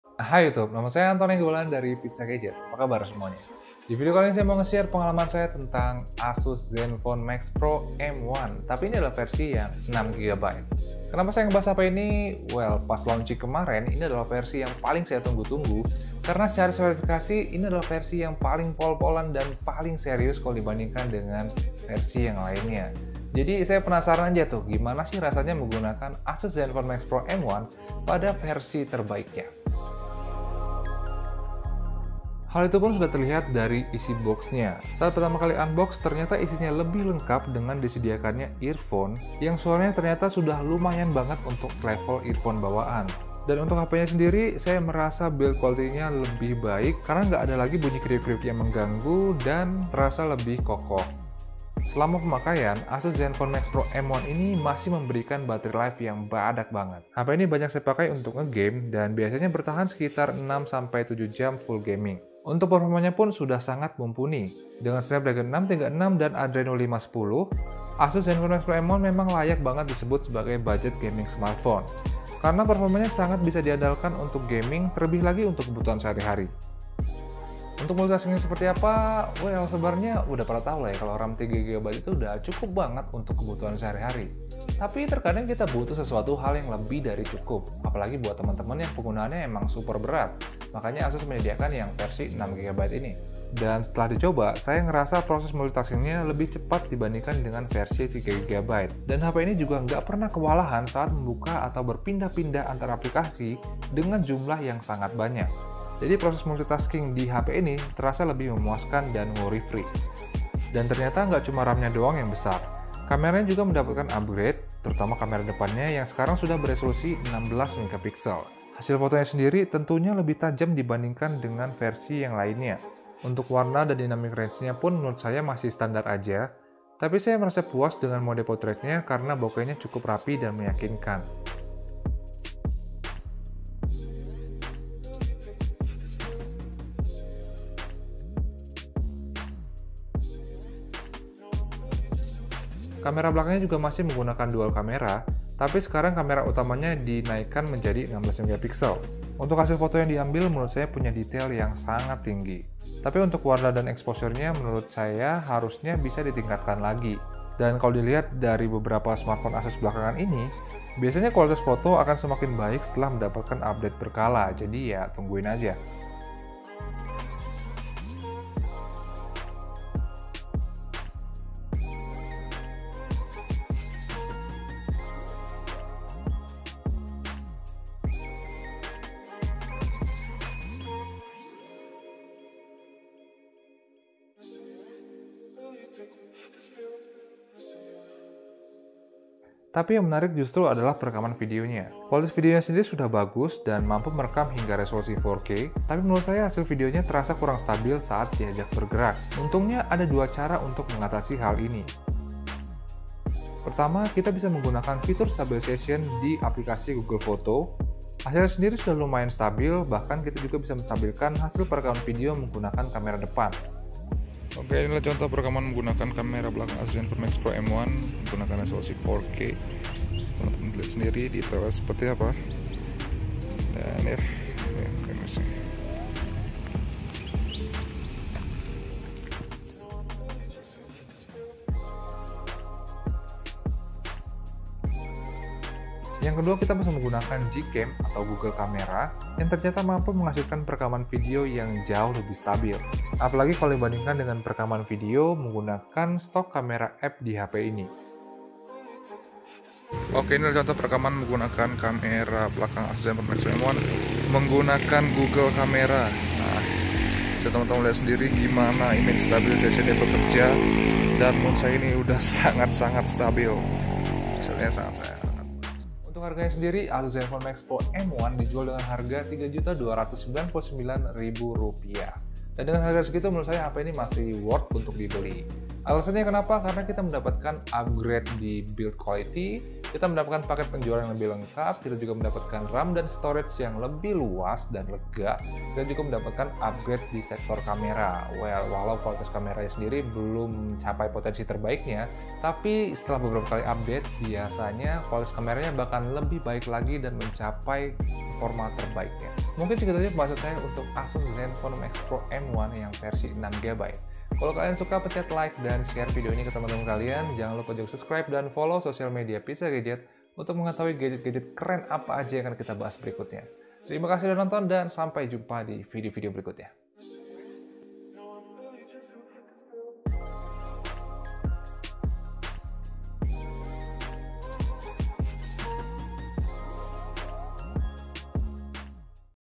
EIS yang Tersembunyi??- Asus Zenfone Max Pro M1 6GB
HI

Spesifikasi lengkap
Asus Zenfone Max Pro M1 Versi Tertinggi
Snapdragon 636
Ram 6GB
Storage 64GB
Kamera 16+5MP
Kamera Depan 16MP
Layar 5,99" FHD IPS
Batre 5000mAH

 Kalo ada saran dan kripik, comment di bawah ya. Thanks Follow